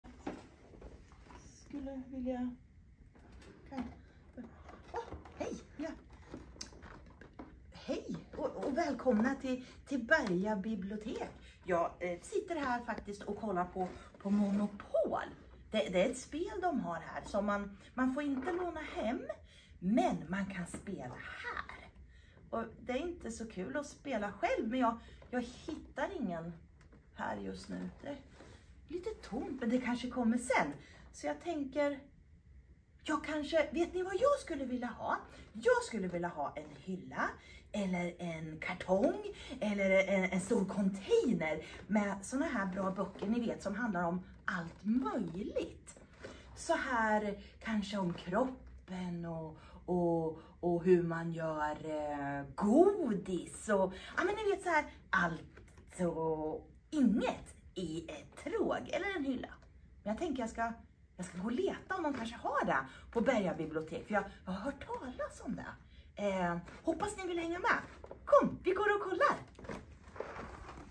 Åsa är på Berga bibliotek
Åsa är på besök hos oss på Berga bibliotek och vill spela monopol men hittar ingen som hon kan spela med. Nu letar hon efter en fantastisk 😍 hylla som hon hört talas om.

Nästa onsdag får du se fortsättningen…